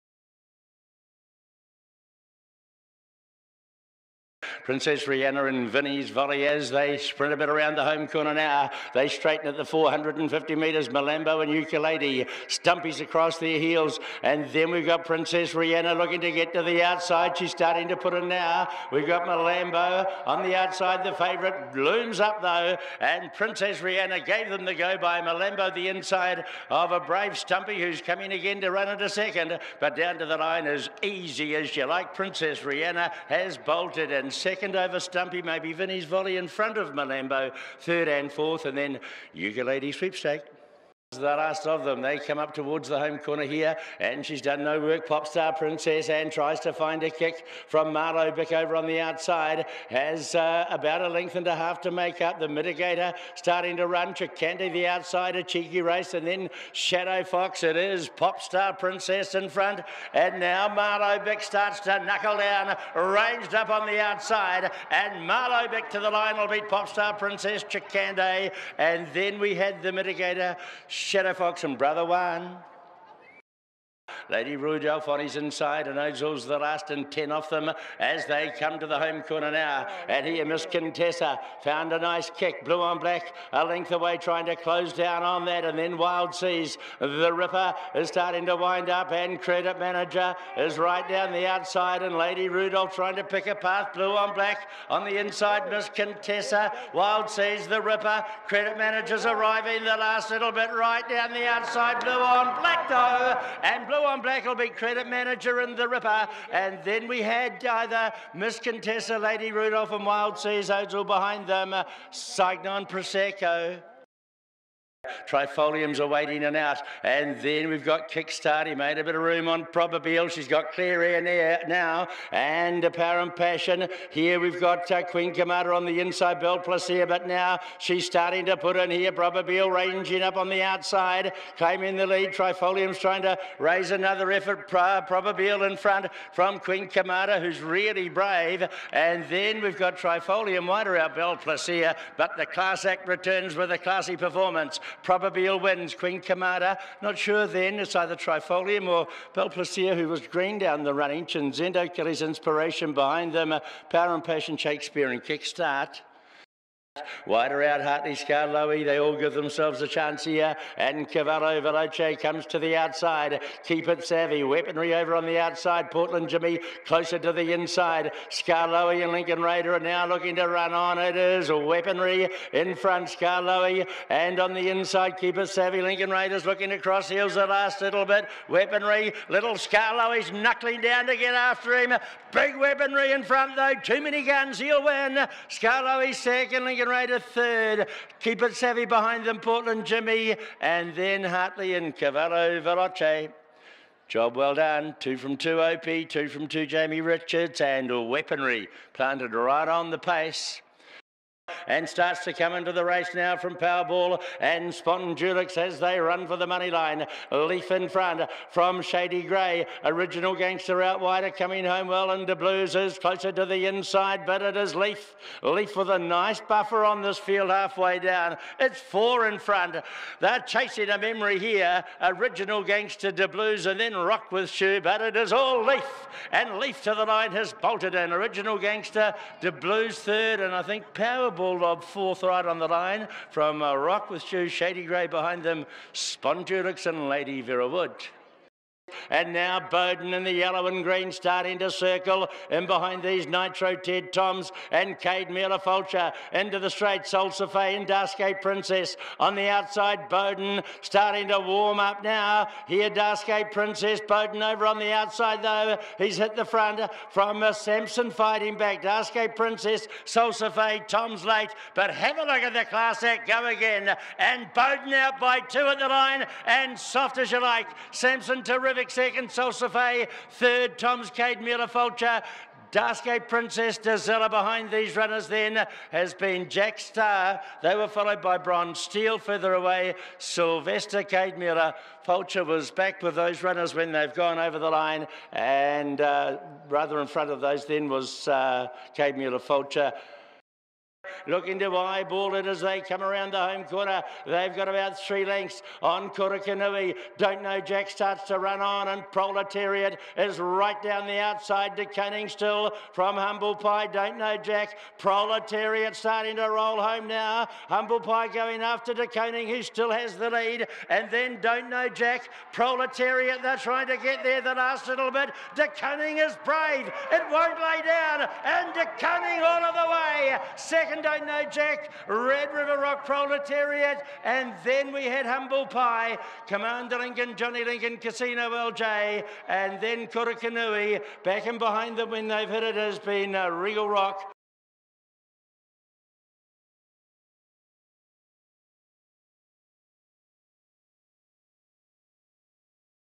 Final 400s: Marton JC @ Awapuni 4 Jan
Watch the final 400s of the 8 races on the card at Awapuni from Saturday 4th January.